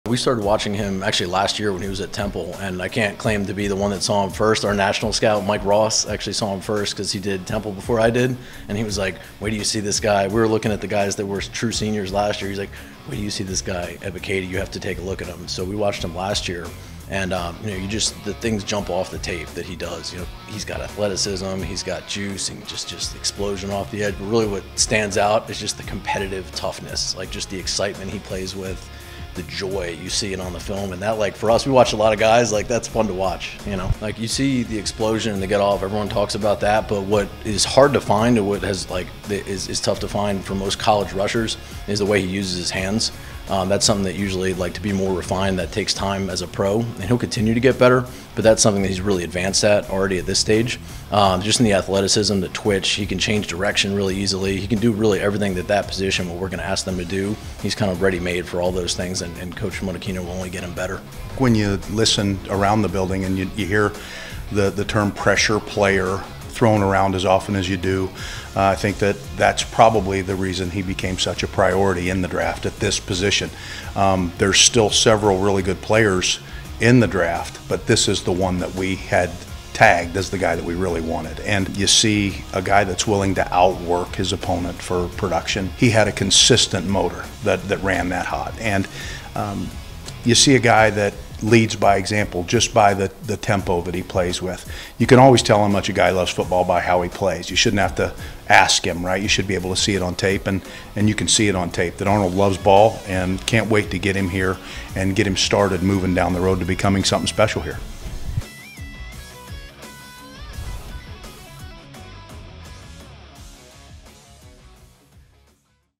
Why the Atlanta Falcons picked outside linebacker Arnold Ebiketie in 2nd round of the 2022 NFL Draft
Outside Linebackers Coach Ted Monachino and area scout Ryan Doyle break down why the Atlanta Falcons traded up to select Arnold Ebiketie with the 38th overall pick of the 2022 NFL Draft.